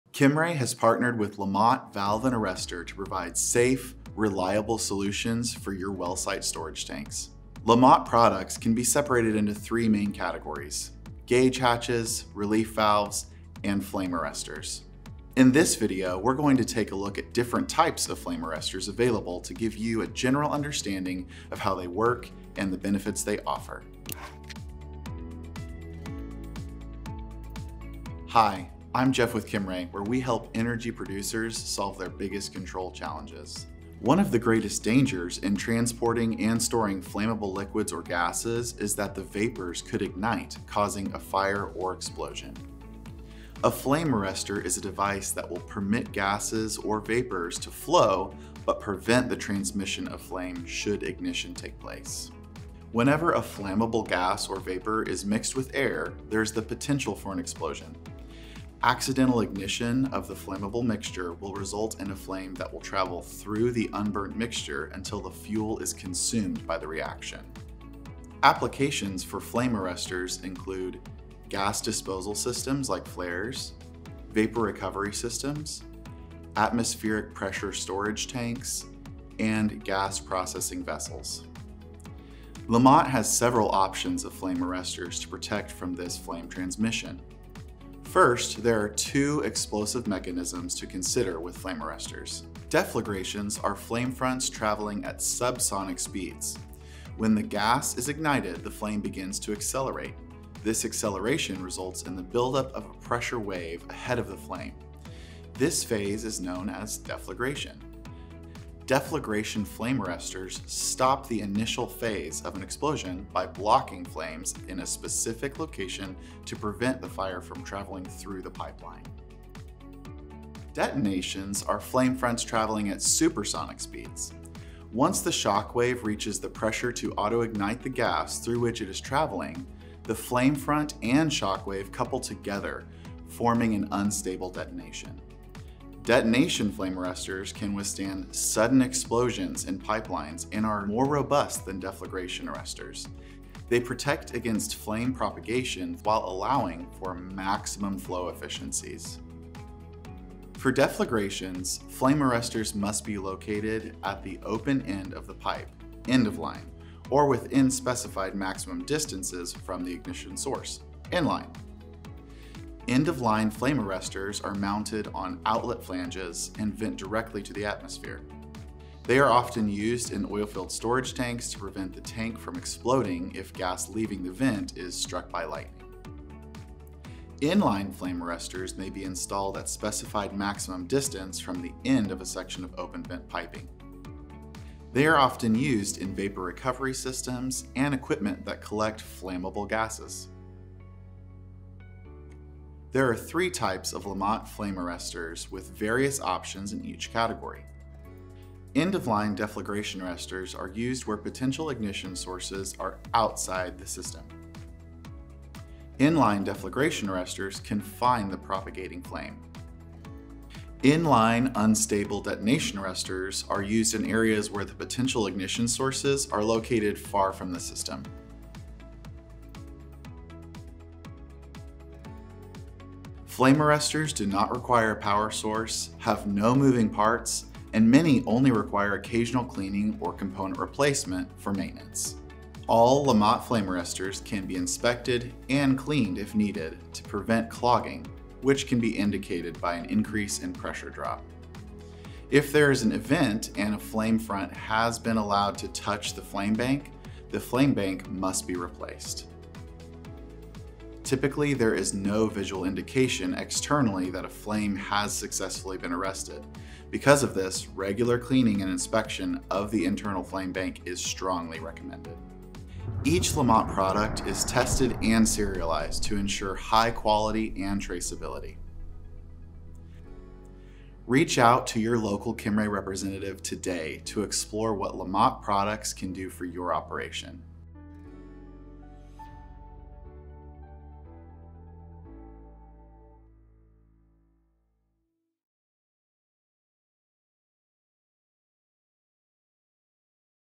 Kimray & LaMOT | Flame Arrestor Types Explained
TIMESTAMPS:
0:00 Arrestors & Applications
1:28 Deflagration vs Detonation
2:30 End-of-Line vs In-Line
3:43 Maintenance & Testing